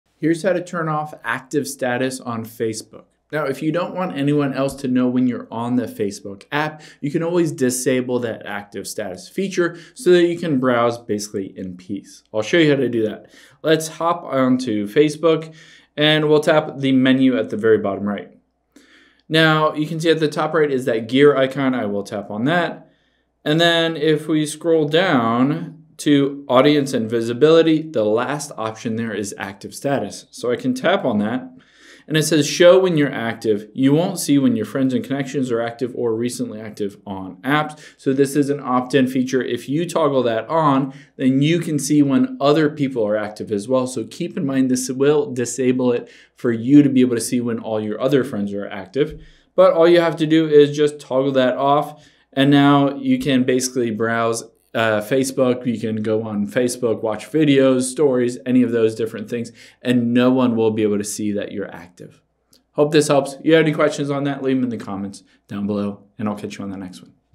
How To Turn Off Active Status On Facebook
Let's disable the active status on Facebook so no one will know if you're online on Facebook or not.